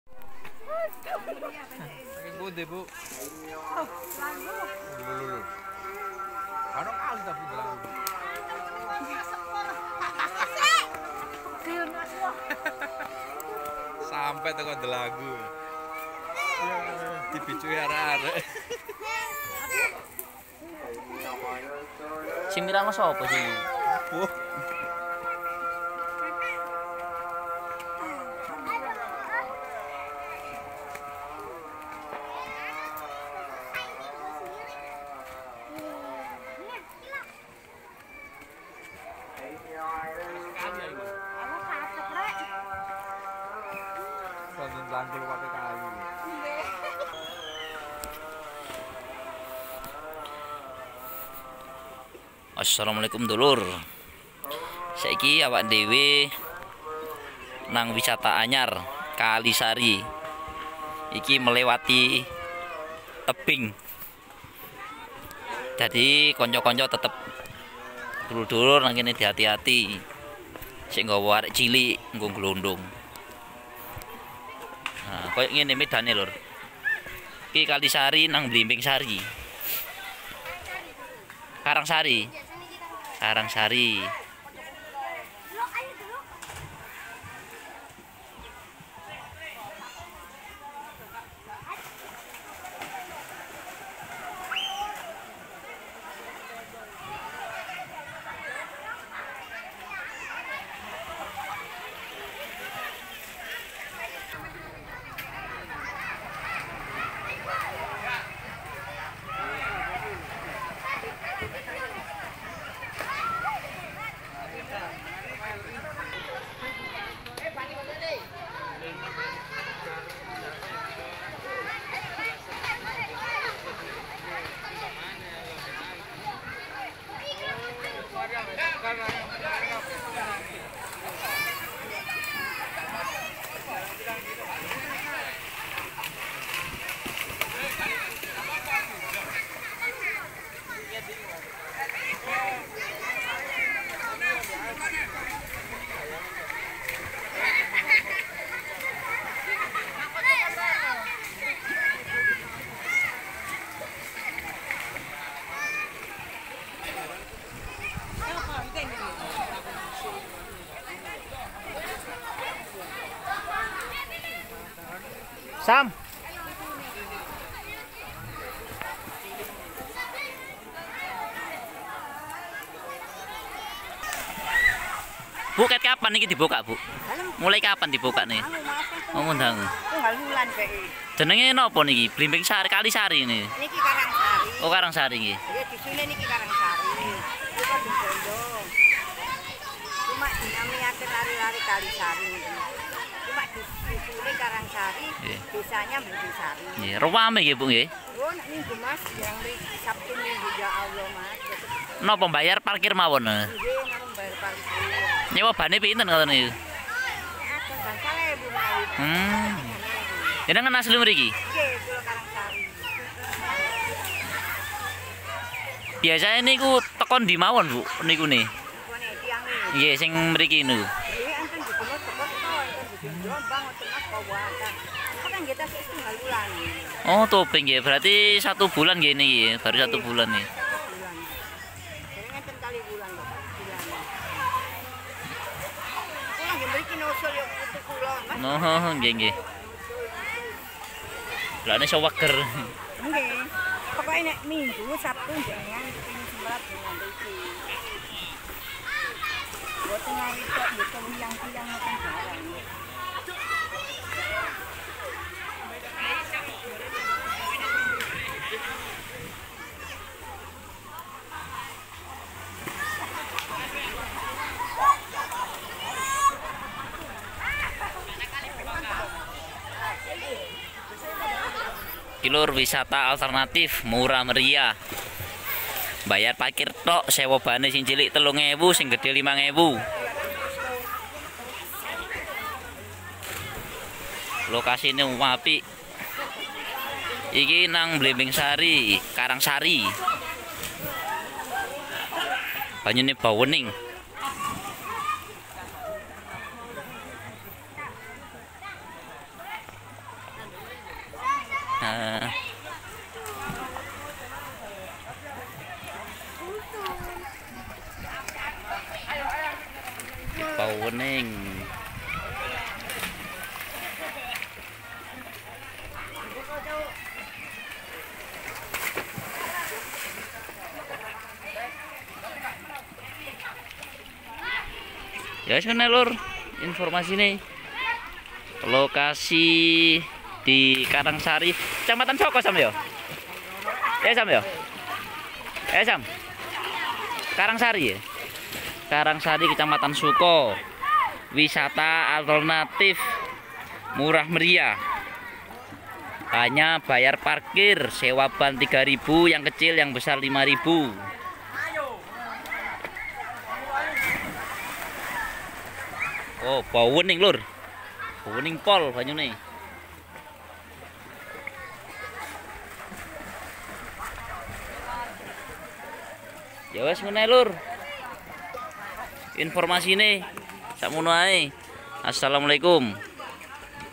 WISATA BARU YANG TERSEMBUNYI KALISARI SOOKO MOJOKERTO
Wisata air untuk bluron airnya bening jernih